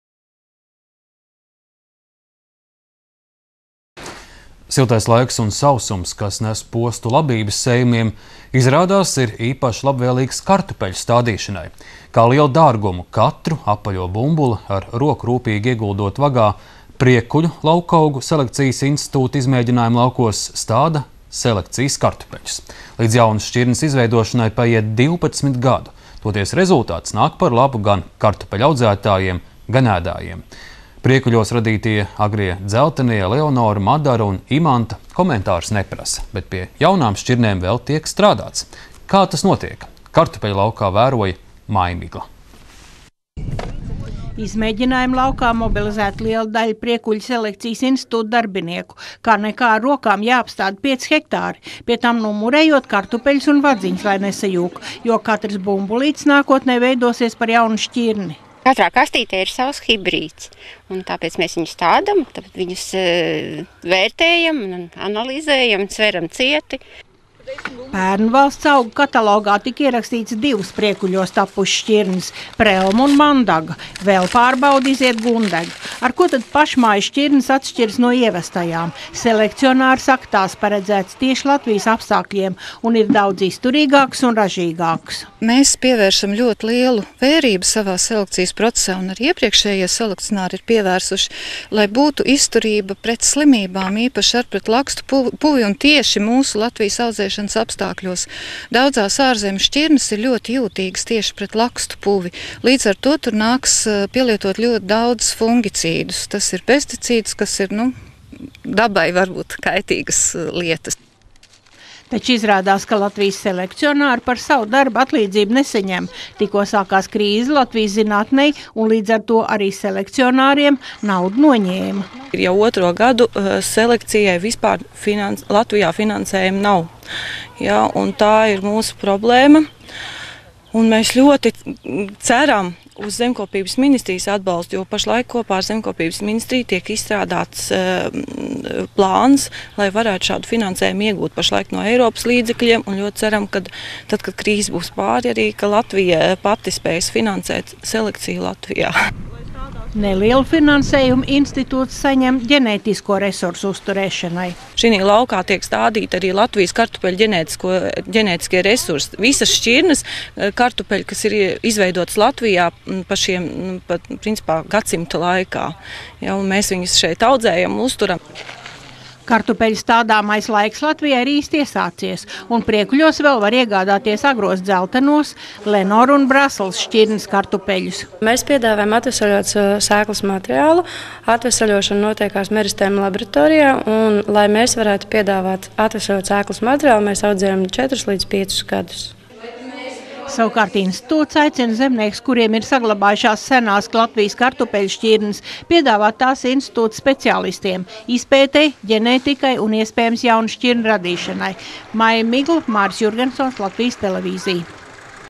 Priekuļos veido jaunas kartupeļu šķirnes
Siltais laiks un sausums, kas nes postu labības sējumiem, izrādās, ir īpaši  labvēlīgs kartupeļu stādīšanai. Kā lielu dārgumu katru apaļo bumbuli ar roku rūpīgi ieguldot vagā, Priekuļu laukaugu selekcijas institūta izmēģinājuma laukos stāda selekcijas kartupeļus. Līdz jaunas šķirnes izveidošanai paiet 12 gadu, toties rezultāts nāk par labu gan kartupeļu audzētājiem, gan ēdājiem. Priekuļos radītie ,,Agrie dzeltenie" , ,,Lenora" , ,,Madara" un ,,Imanta" komentārus neprasa, bet pie jaunām šķirnēm vēl tiek strādāts.